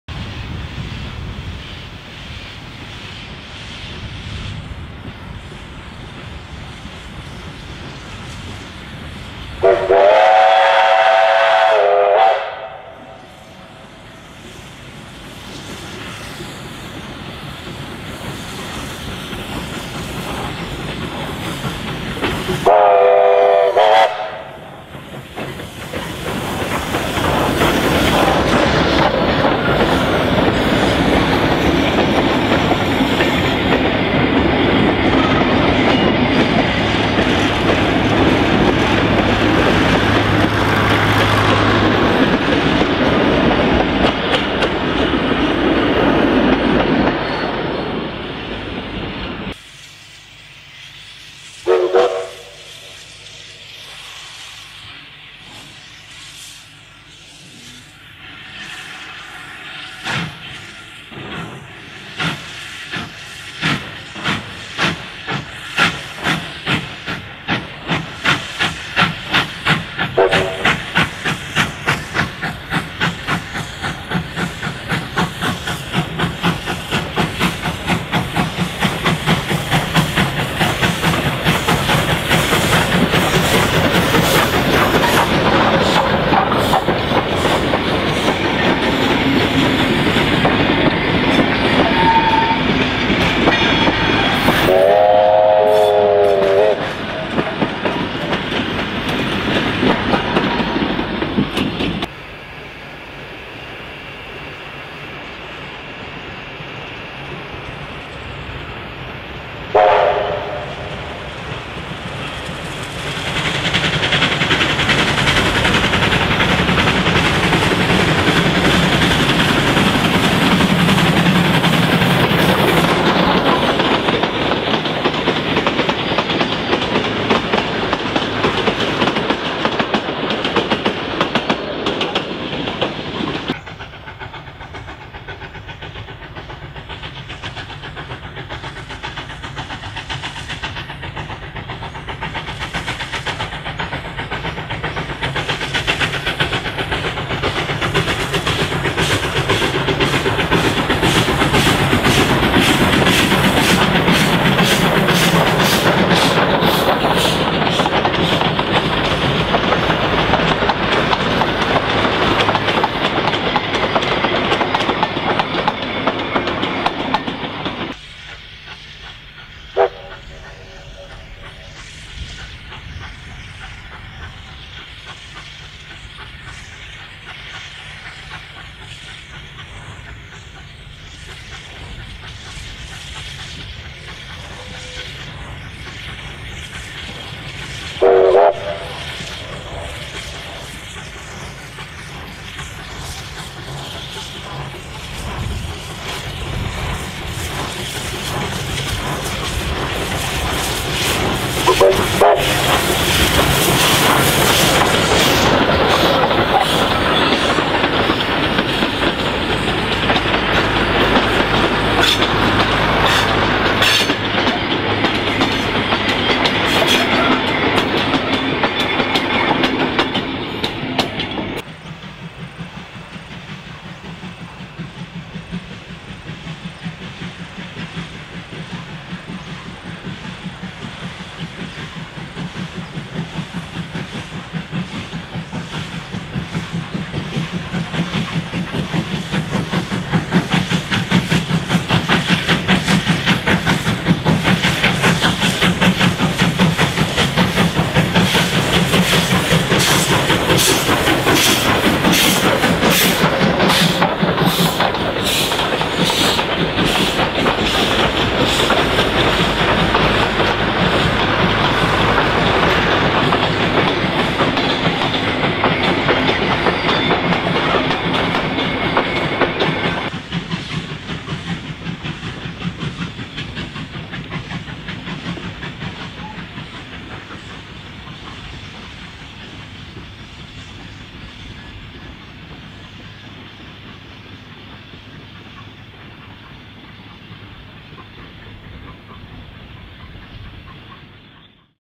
Australian Trains: Victorian Goldfields Railway - Australia Day 2011
On the Australia day public holiday in January 2011, Vulcain foundry built steam locomotive J541 was operating the regular heritage services between Maldon and Castlemaine.

The Victorian Goldfields Railway is a volunteer run organisation that has preserved the 10 mile Castlemaine to Maldon branch line in the central goldfields region of Victoria. Fatures of the line are the timber bridges, 1 in 40 grades and short 22ft 60lb d rail.

This video features lineside shots of the morning run starting near Maldon, following the train to Castlemaine and back to Maldon